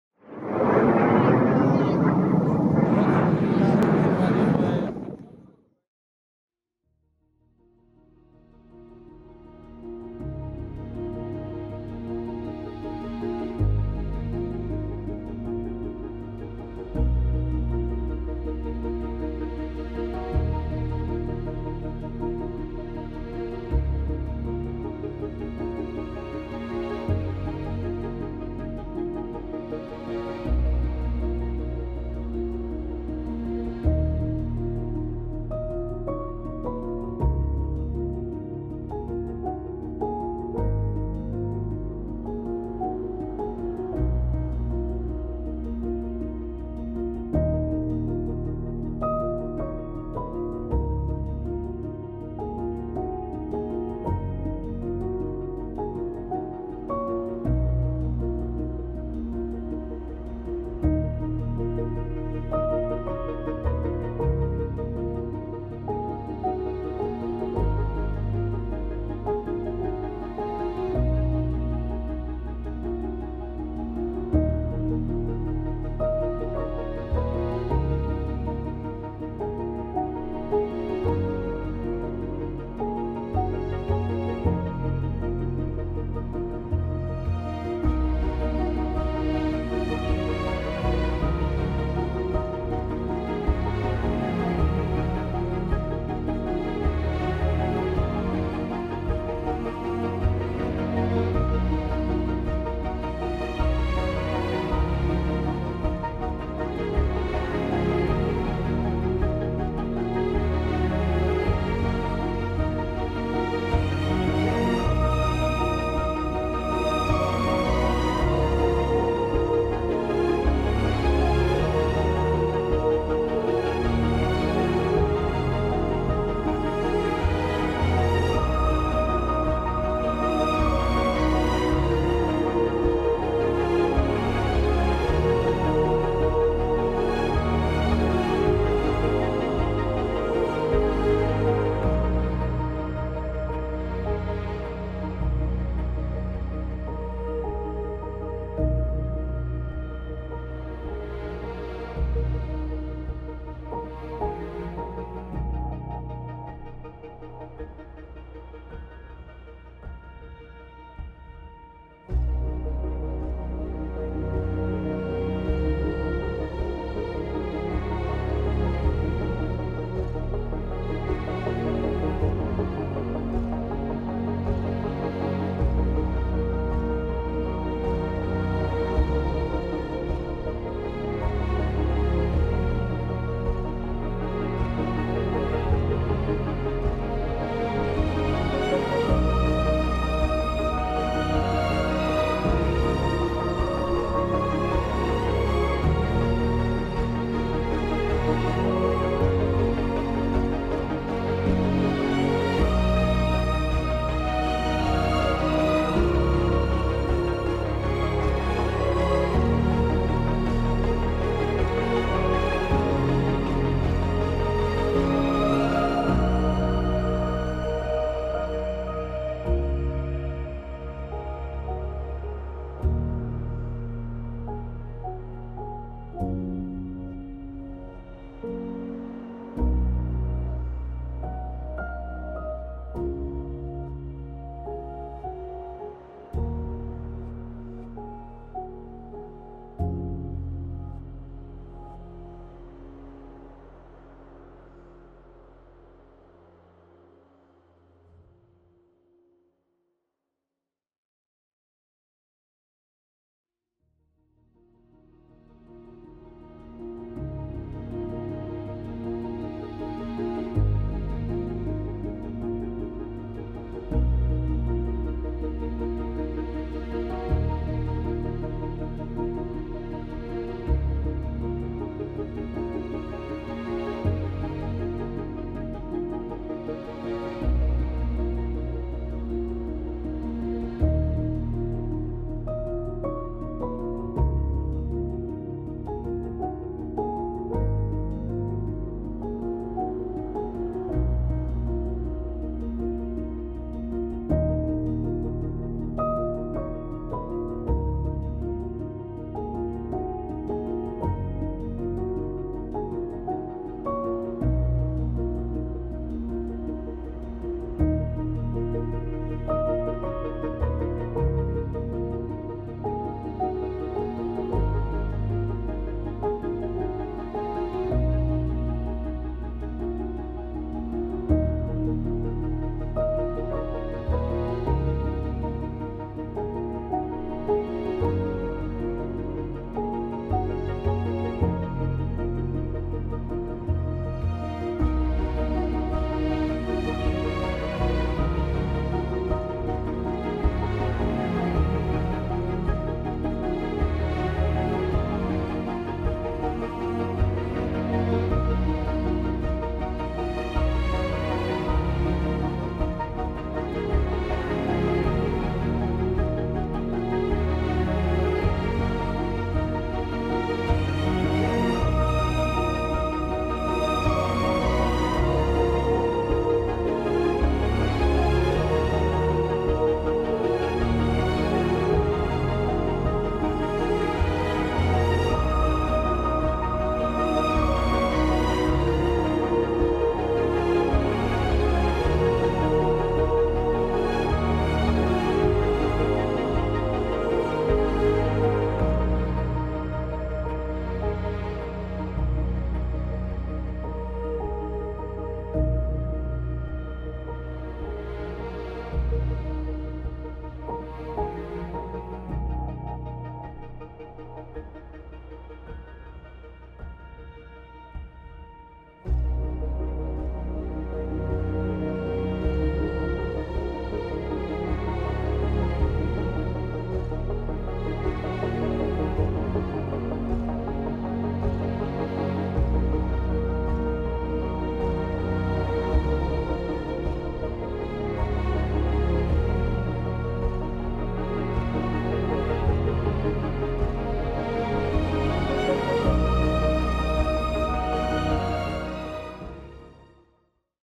Prikaz i obilazak novog sistema za radio-elektronsku borbu "Repellent-1" vojske Srbije, Niš 2024.
Thumbnail credit: /